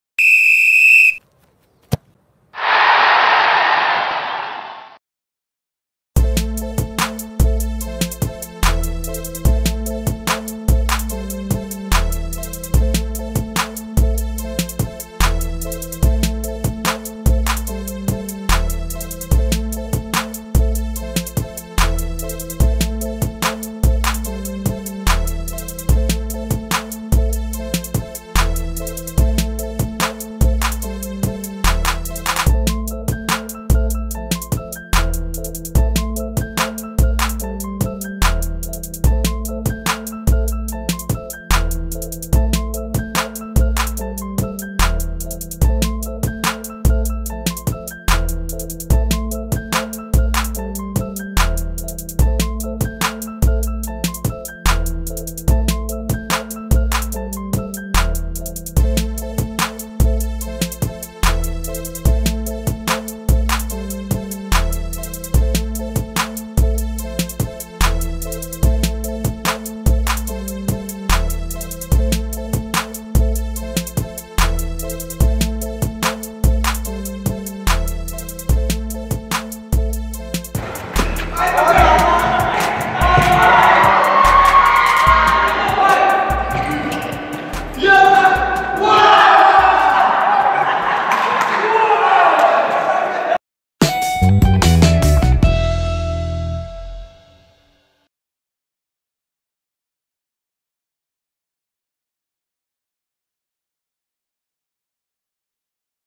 Futsal au Gymnase des Burgondes
Futsal tous les mercredis après-midi au gymnase des Burgondes à Saint-Julien-en-Genevois.

Pour plus d'infos contactez Micka : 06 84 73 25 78